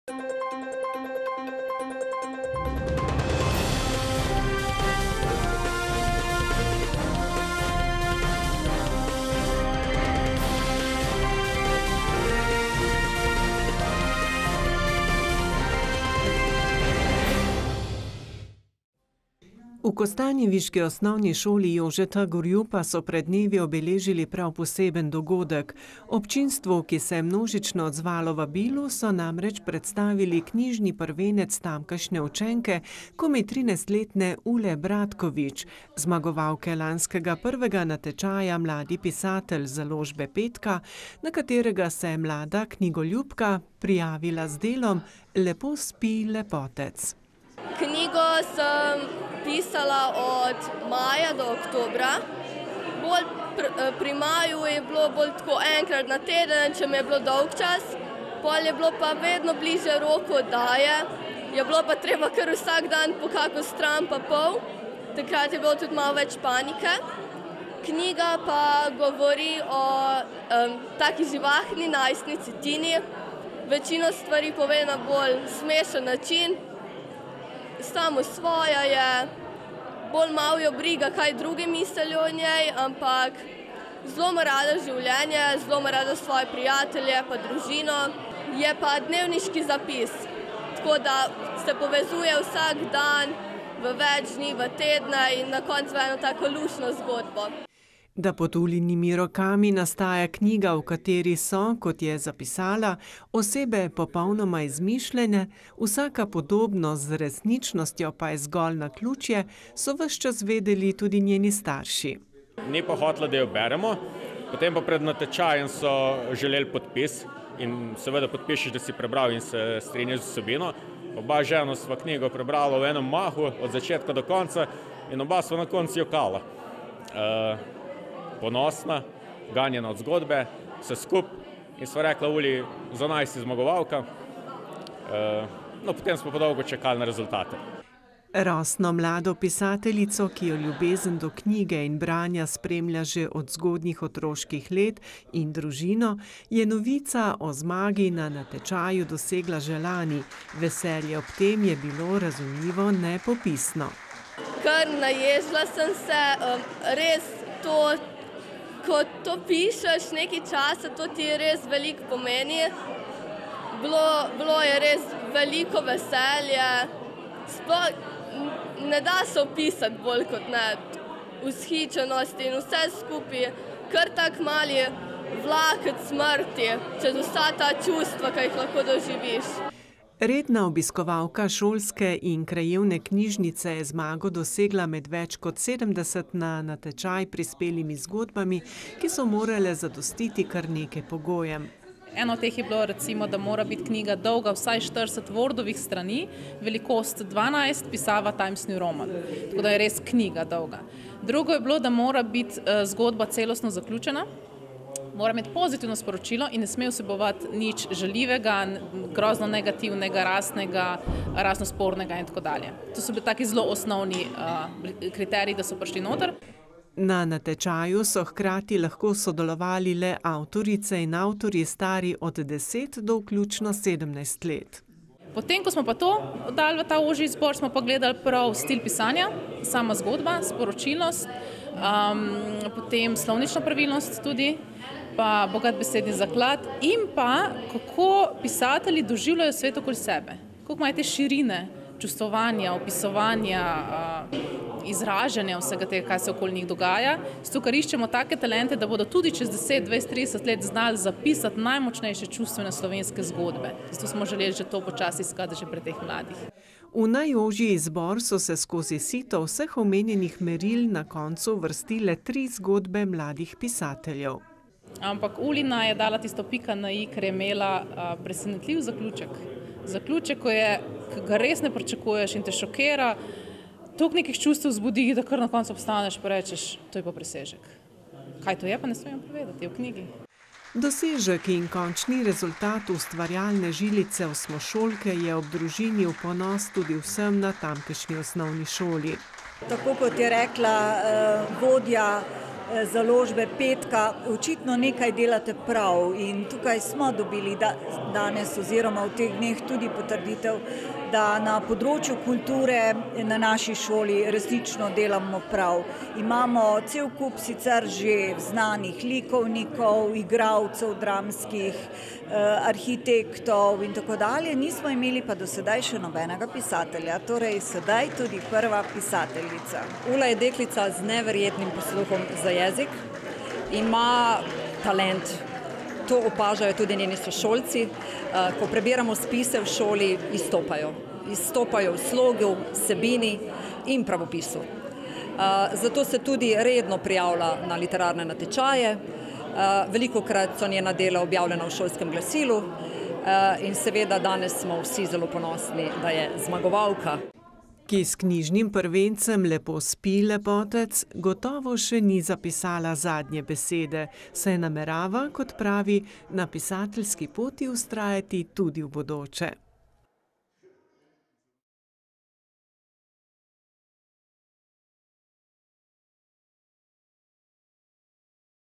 Mlada pisateljica Ula Bratkovič
KOSTANJEVICA NA KRKI - V kostanjeviški osnovni šoli Jožeta Gorjupa so pred dnevi obeležili prav poseben dogodek. Občinstvu, ki se je množično odzvalo vabilu, so namreč predstavili knjižni prvenec tamkajšnje učenke, komaj 13-letne Ule Bratkovič, zmagovalke lanskega prvega natečaja mladi pisatelj založbe 5ka, na katerega se je mlada knjigoljubka prijavila z delom Lepo spi, lepotec.